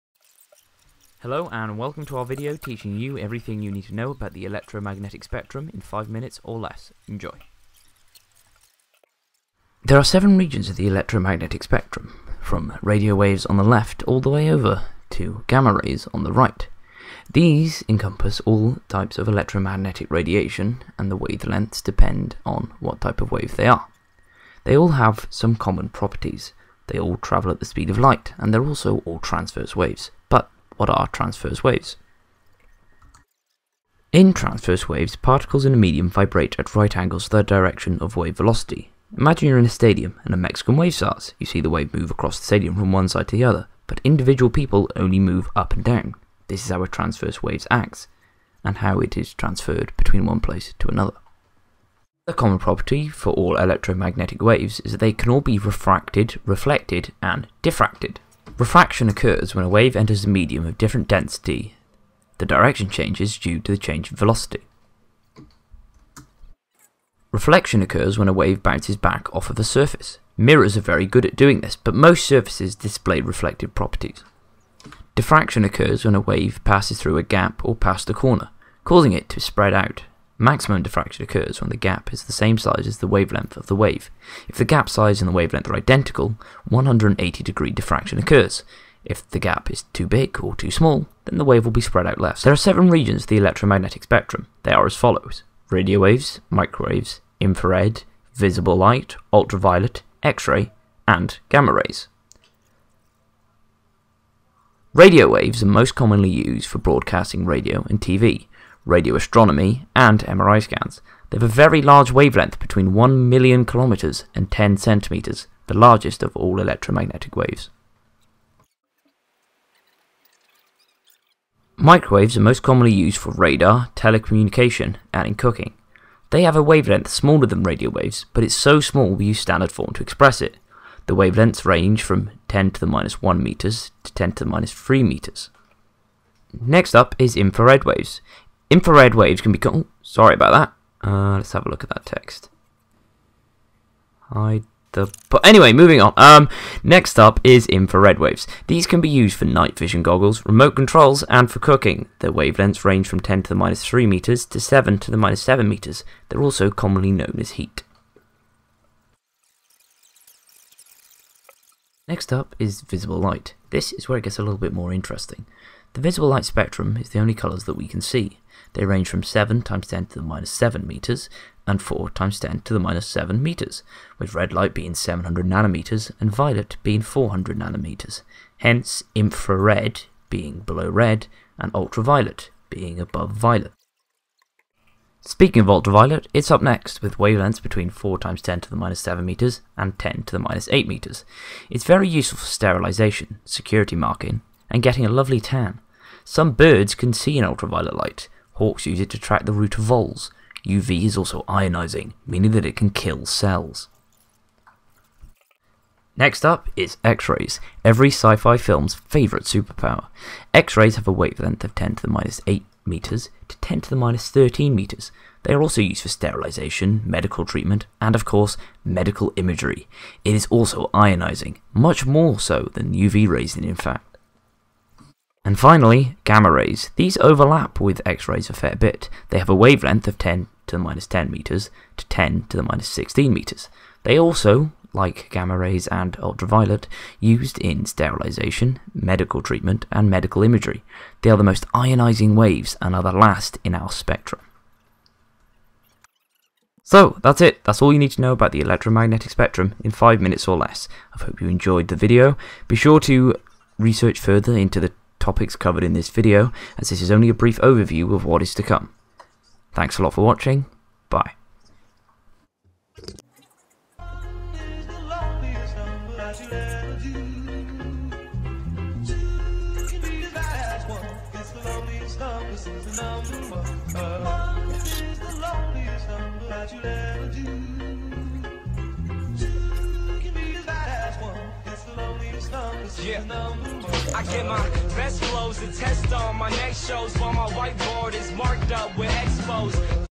Electromagnetic Spectrum Explained
The electromagnetic spectrum explained. Featuring properties and uses of each region of the spectrum. Filmed on a GoPro. Made for a school project and is useful for the OCR Physics AS level Electrons, Waves and Photons exam.

Please visit the people who helped me make this video.

Possibly the least extreme use for a GoPro ever.